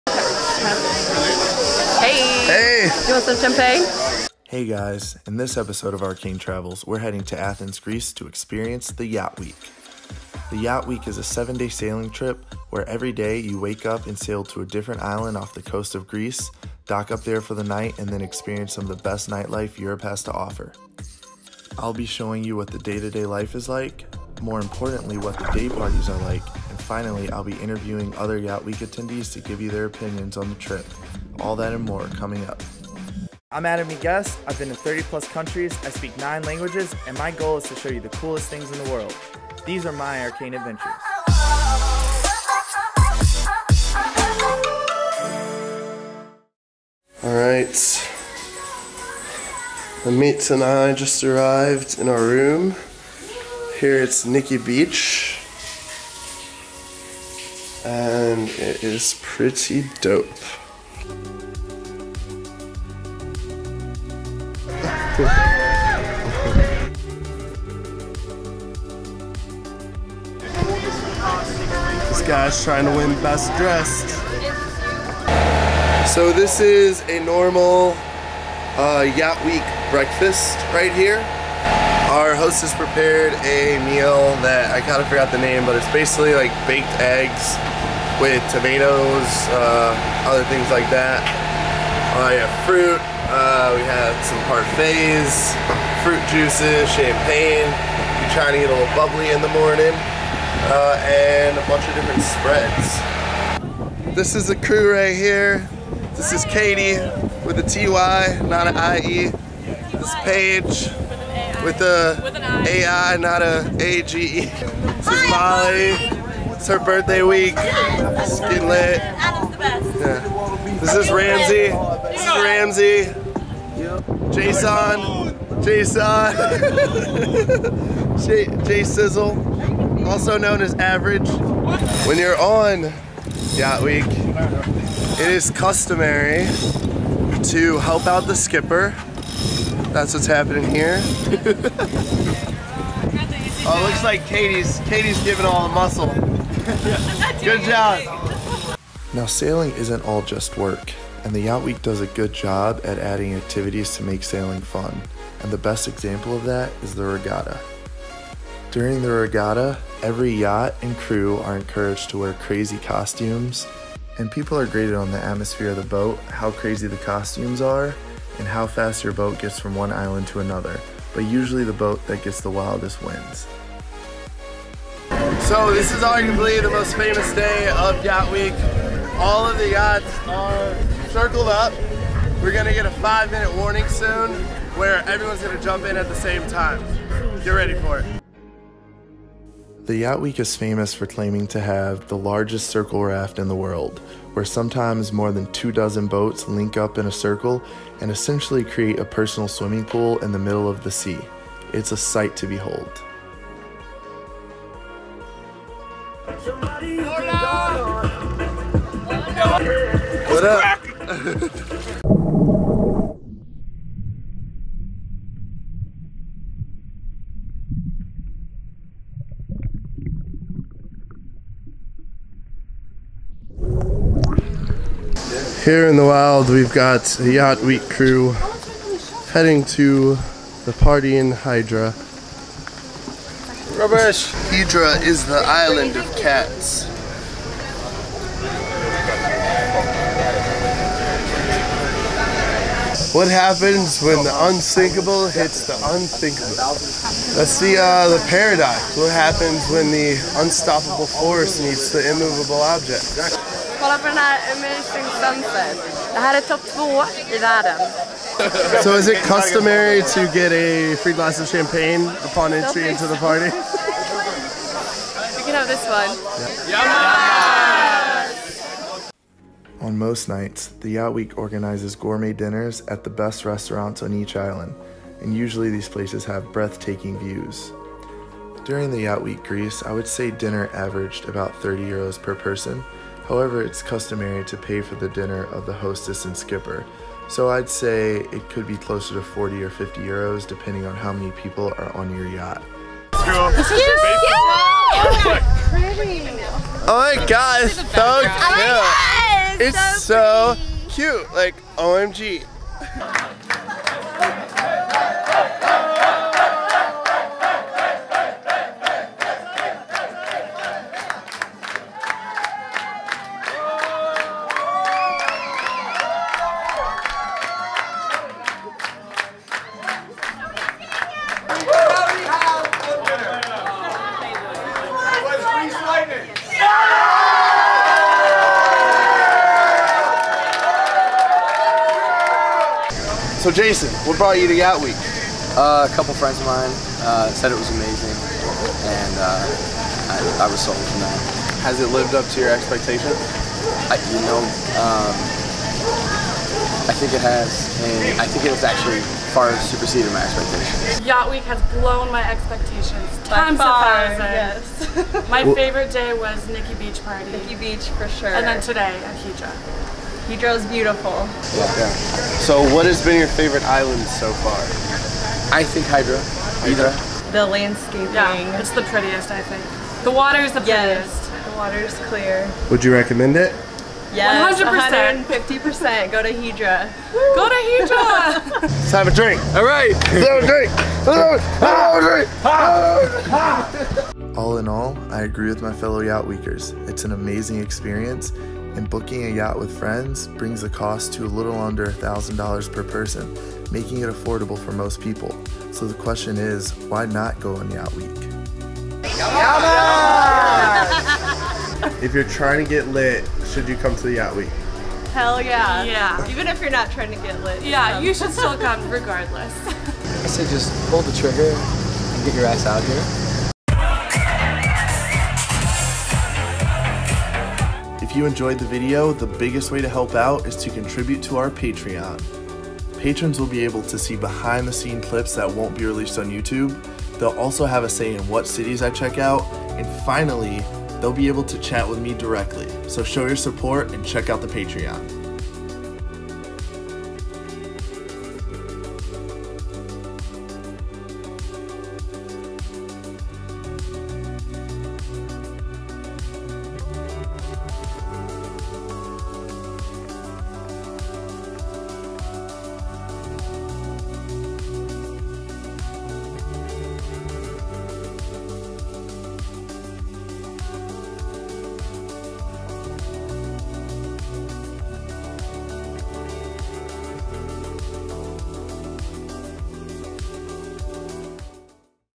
The Yacht Week - Everything You Need To Know! Greece, Croatia, etc.
In this episode of Arcane Travels, we're going to Europe to attend the Yacht Week! Everything from what to eat, what to wear, what parties you'll attend, how much certain things cost and what the vibes are in general; this is the video to watch to learn about the Yacht Week from the perspective of a normal crew attending the event.

While we will focus on the Greece Route in this episode, I've attended the Yacht Week Croatia Route as well and they operate very similarly. So watching this video will let you know what to expect for both routes. Get ready to set sail!

Video Editor: Bassey Arikpo
Slow Mo Vids: Ramzi Dreessen